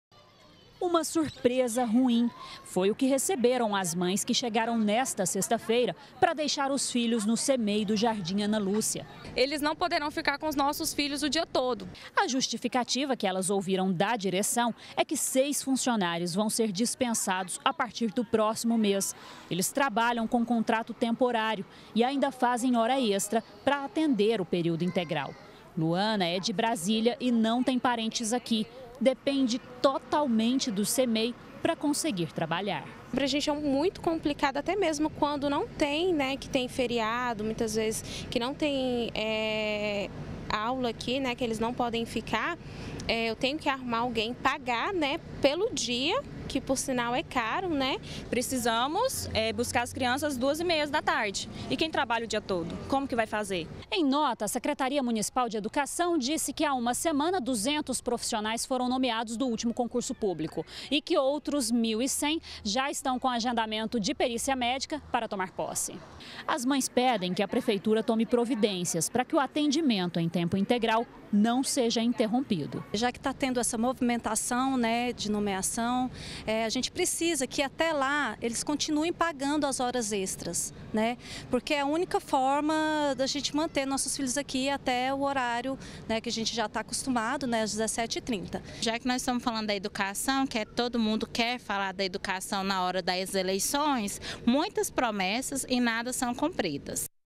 GR - Creche: Crianças serão dispensadas mais cedo - 27-04-2018
CMEI do Jardim Ana Lúcia vai deixar de funcionar em período integral. As crianças vão ser liberadas às 2 e meia da tarde. As mães reclamam que a mudança vai dificultar e, muito, já que a maioria trabalha até às 6 da tarde.